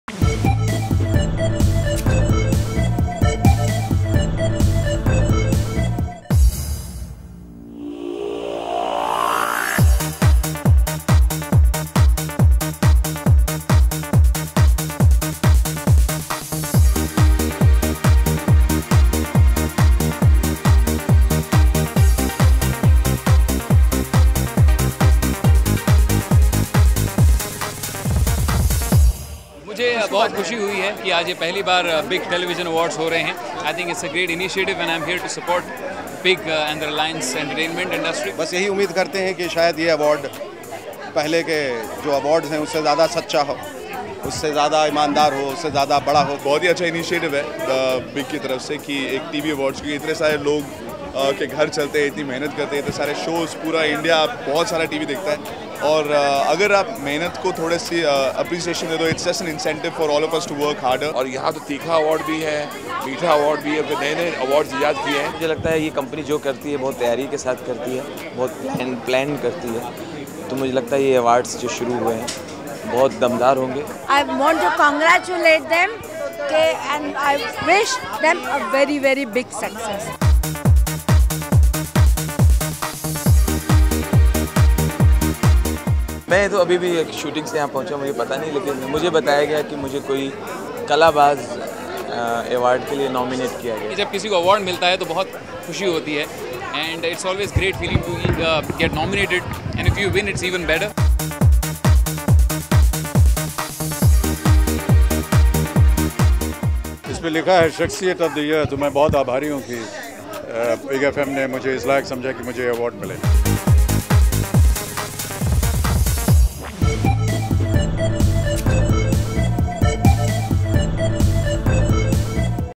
Celebrities at the BIG Television Awards!
Amitabh Bachchan, Ravi Kishen, Minnie Mathur, Shekhar Suman, Ragini Khanna, Ranvijjay, Ishq Bector, Dolly Bindra, Anu Malik, Aruna Irani, Raju Shrivastav, Rahul Vaidya and others at the BIG Television Awards.